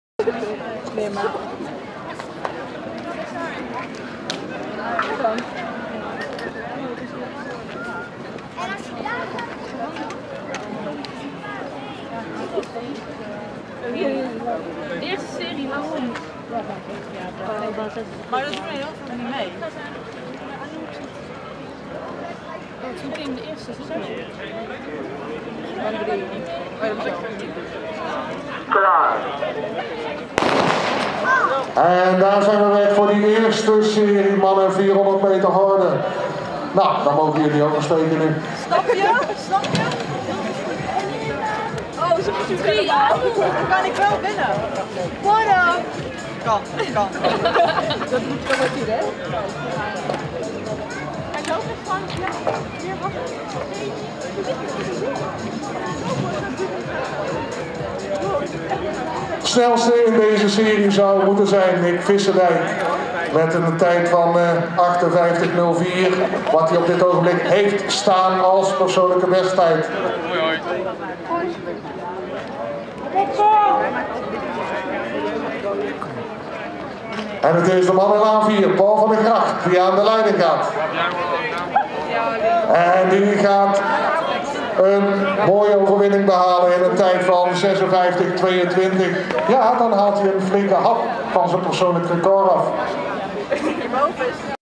Losse Veter T-Meeting - 400mh Mannen - Serie 1
1. Paul v.d. Gragt - Hylas - 56.20
2. Marcel Roosen - Eindhoven Atletiek - 58.10
3. Niek Visschedijk - THOR - 58.43
4. Bas Land - Pijnenburg - 58.94
5. Beer Lammers - Fit - 59.20
6. Jian Ye - Attila - 1:08.09